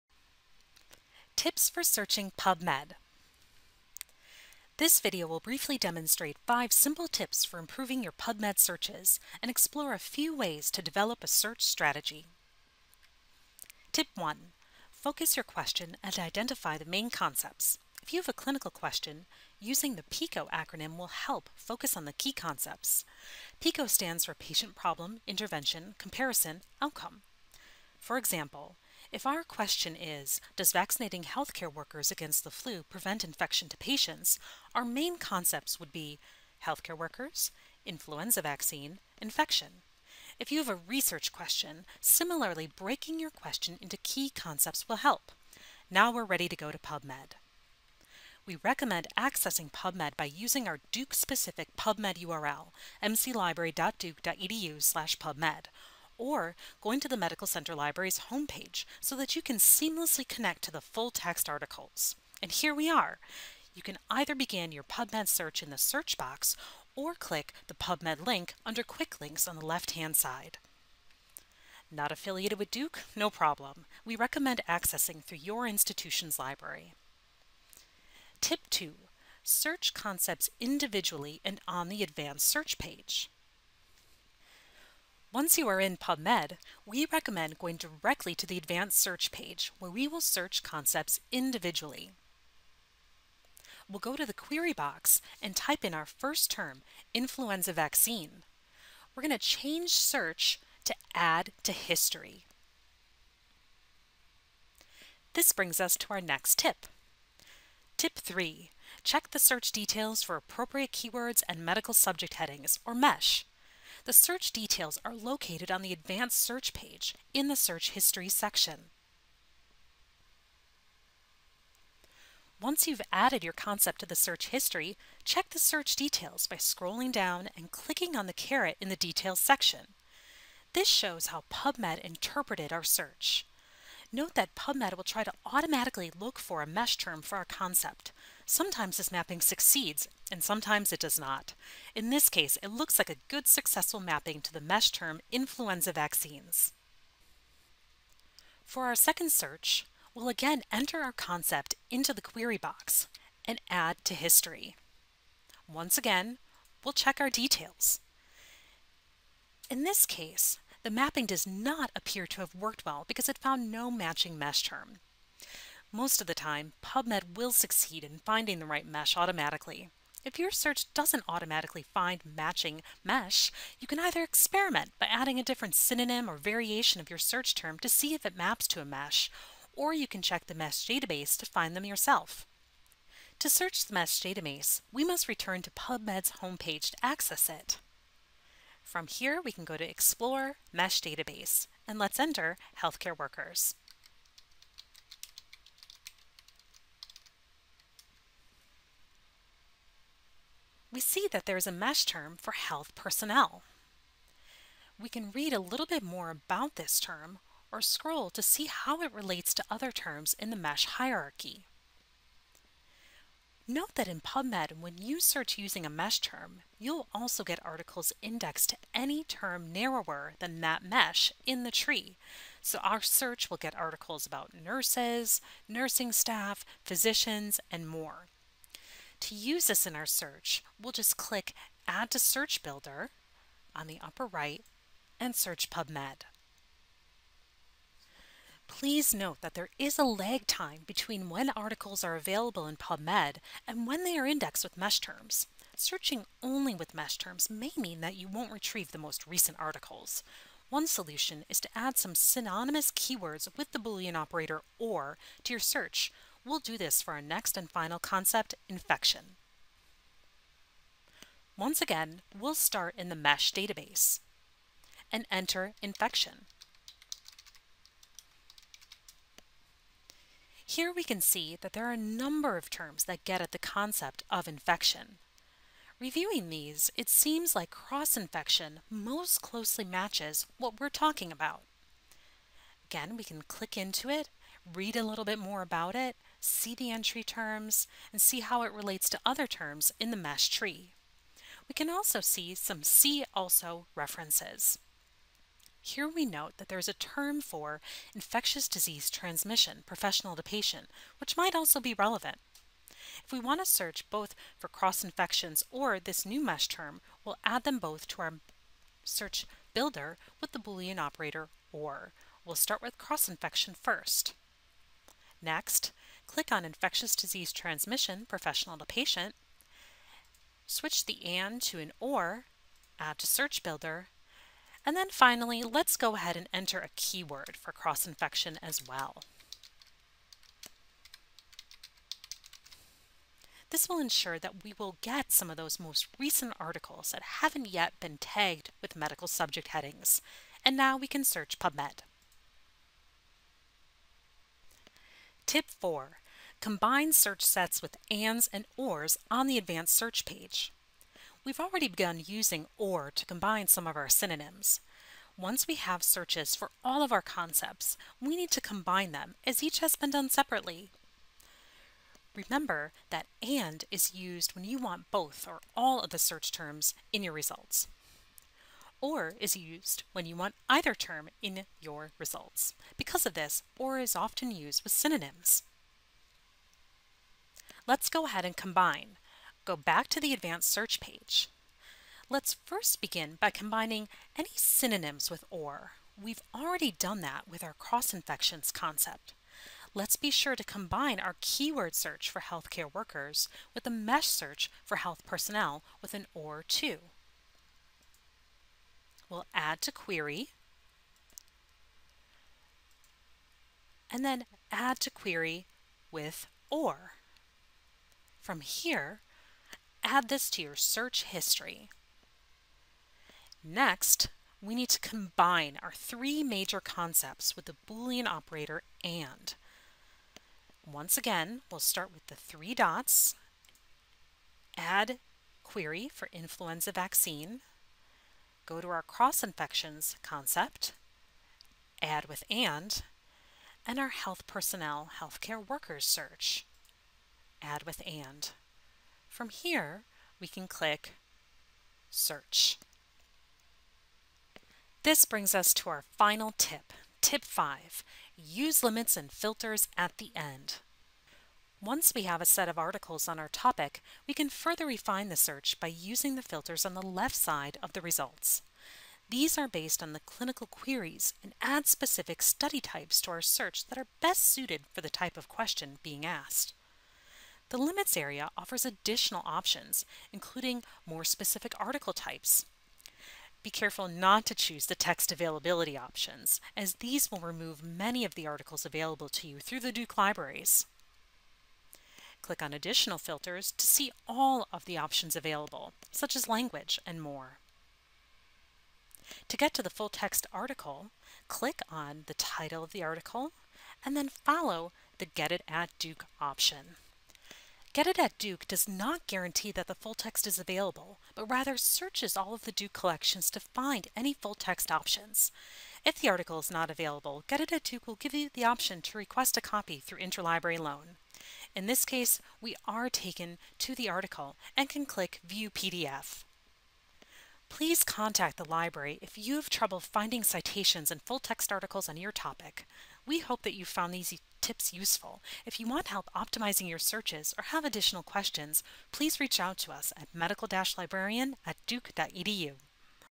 Tips for Searching PubMed
This video demonstrates five simple tips for improving your PubMed searches and explores a few ways to develop a search strategy.